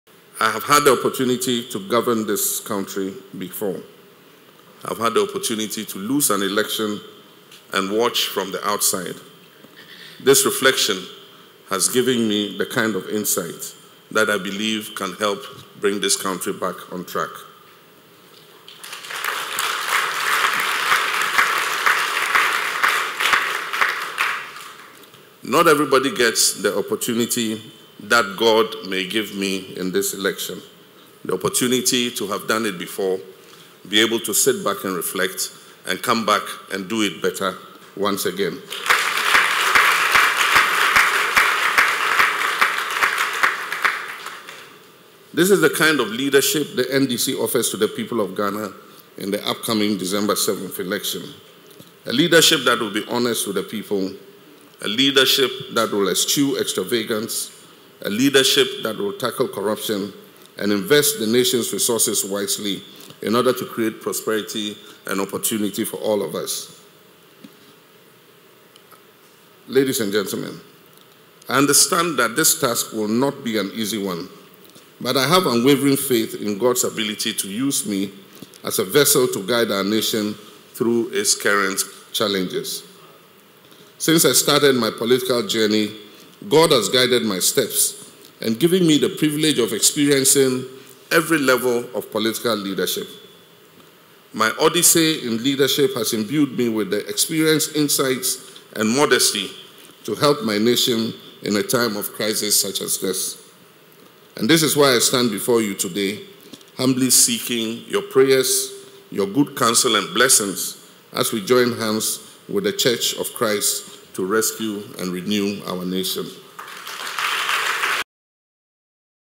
The NDC Leadership Will Tackle Corruption
The NDC will offer a leadership that will tackle corruption and invest the nation's resources wisely... - John Mahama